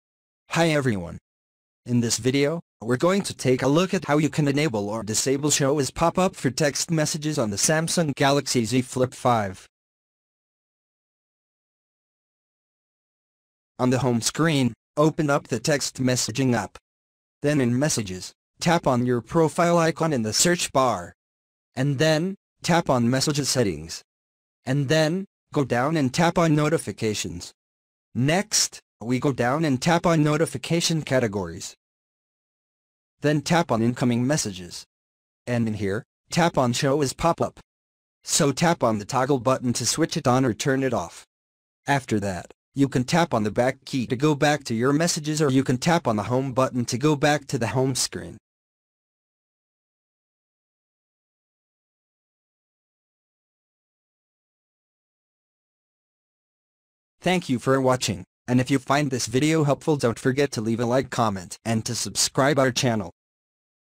Samsung Galaxy Z Flip 5: How to Enable/Disable Show As Pop-up for Text Messages
Learn how you can enable or disable Show as Pop-up for Text Messages on the Samsung Galaxy Z Flip 5.